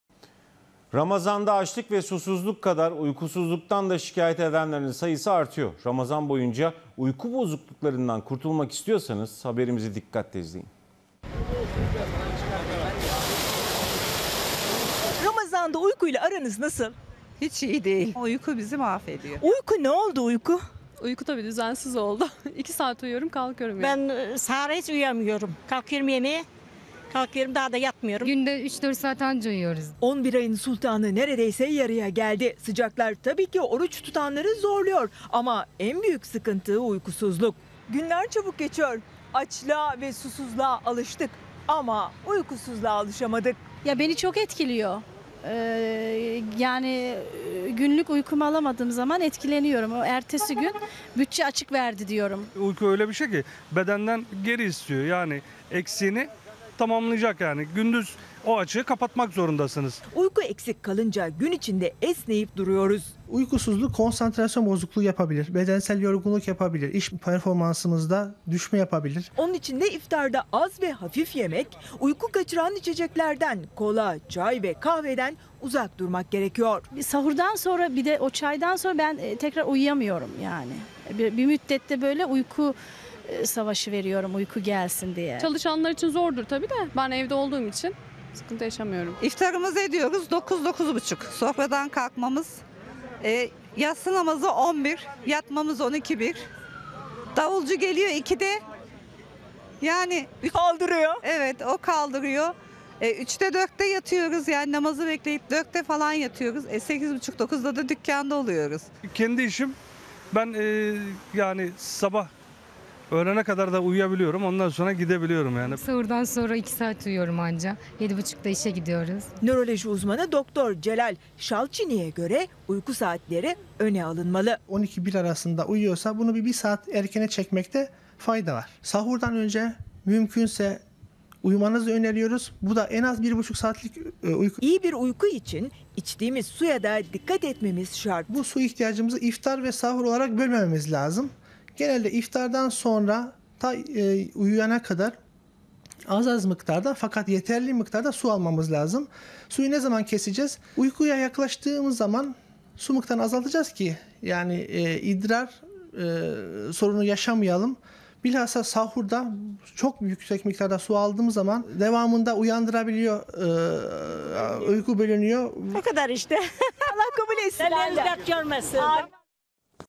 Ramazanda uyku sorunları
Nörolog Uzm. Dr. Celal Şalçini

Kanal D